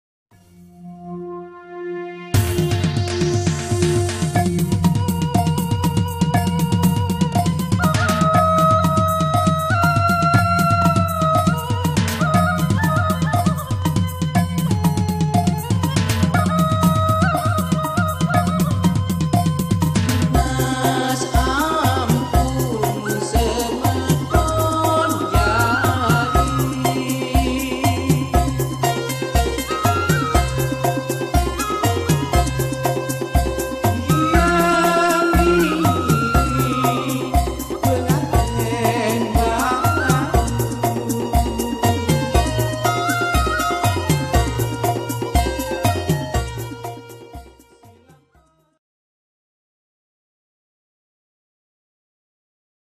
Selusur Nusantara Eksplore Indonesia (Adat Presian Suku Sasak Lombok - NTB)
Selusur Nusantara Eksplore budaya dan wisata beserta kearifan lokal .
part one, 2015  Banyuwangi, Bali, NTB, NTT, Papua

check our web and Instagram g for detail activity